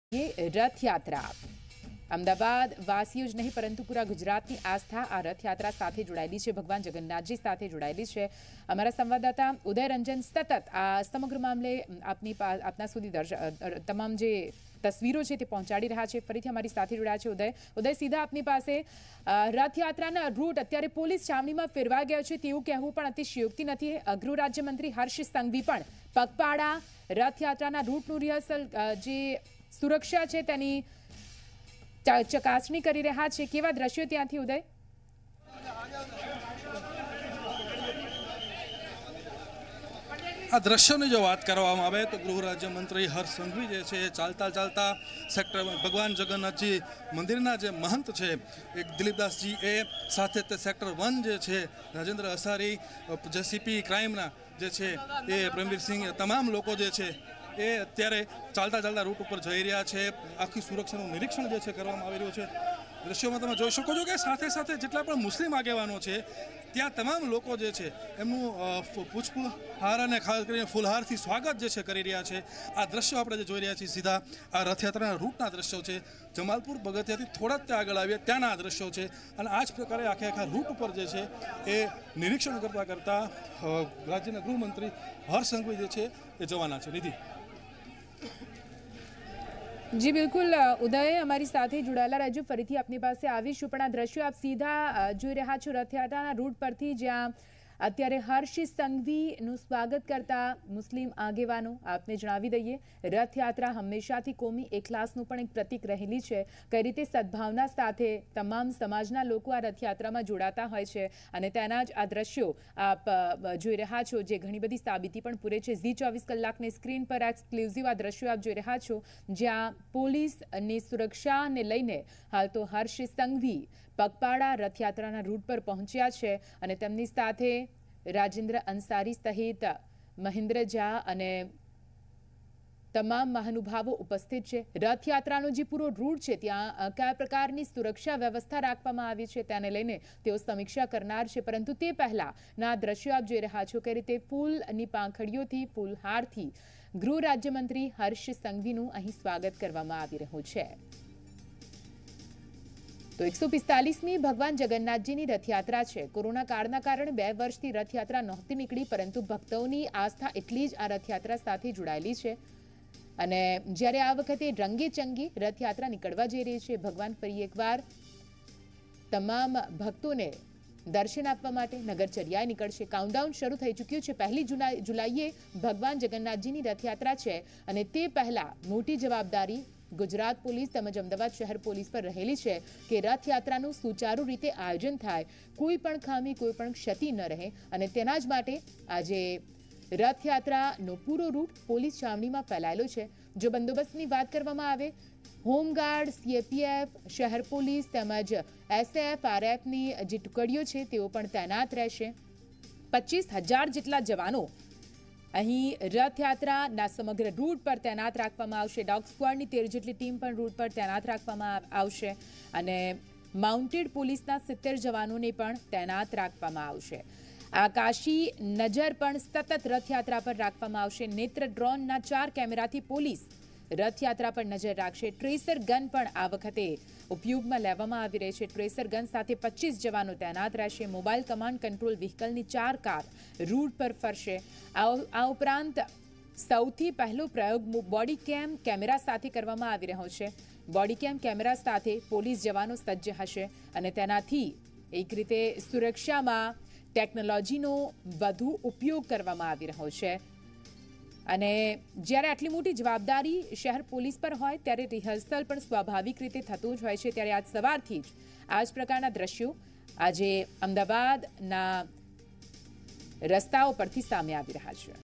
Gujarat HM Harsh Sanghvi conducts foot patrolling on Rathyatra's route in Ahmedabad | Zee News
અમદાવાદમાં 145મી રથયાત્રા પૂર્વે ગૃહ રાજ્યમંત્રી હર્ષ સંઘવીનું ફૂટ પેટ્રોલિંગ